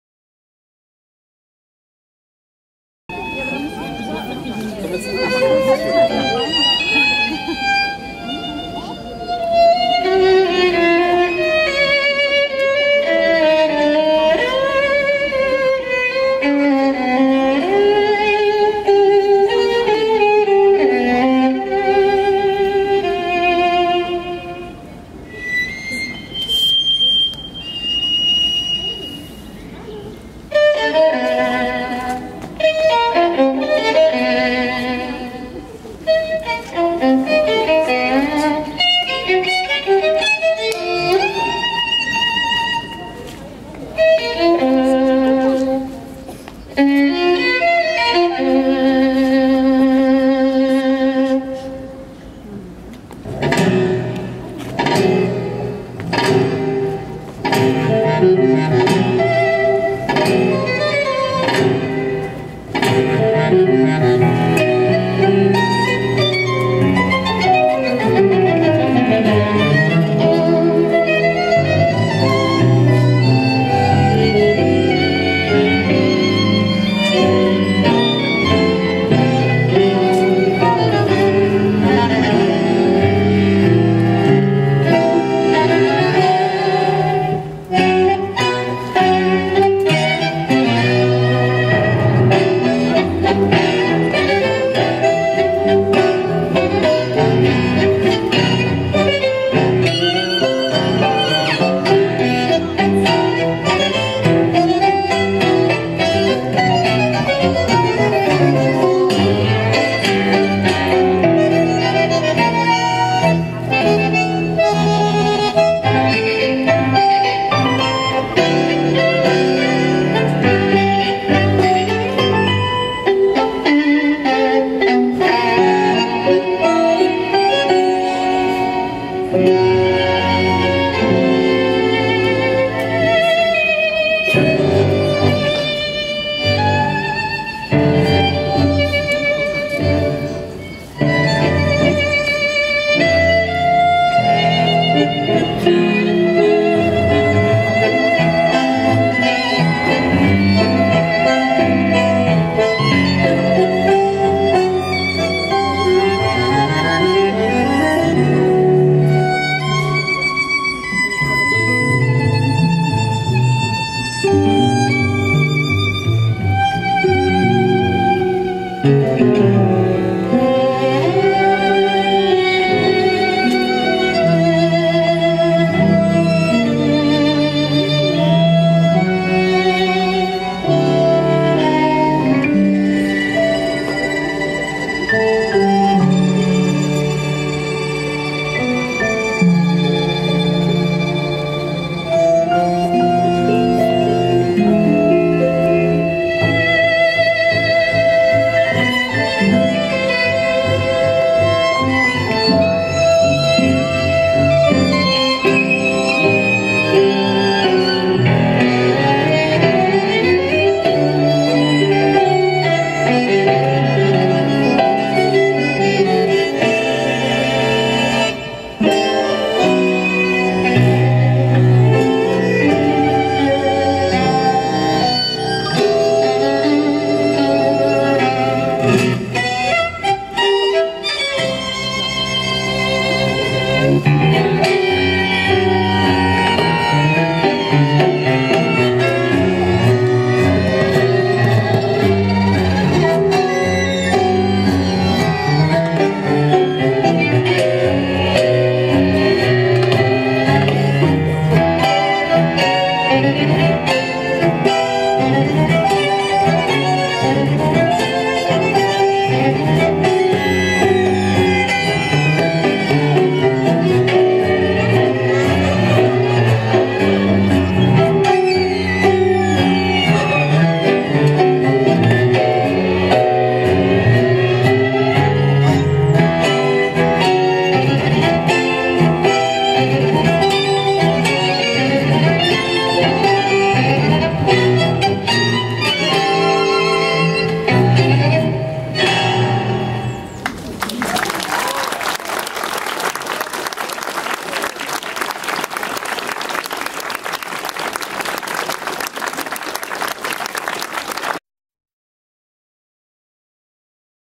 TANGO - International Dance Day 2012 (Muenster Westf. Germany)
Tango!
Dancers: Katharina Szoek & Jens Siebeneicher, Rebel Dance Company, Muenster Germany.
Performance during the 30th Anniversary of International Dance Day (Welt Tanztag), April 29th 2012.